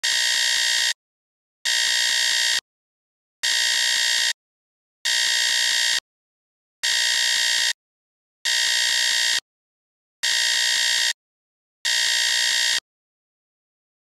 Nova 100 Oran Kontrol Yüksek Basınç Alarmı
Belirlenen maksimum basınç regülasyon değerinin üstünde bir değer okunmuştur, basıncı düşürünüz.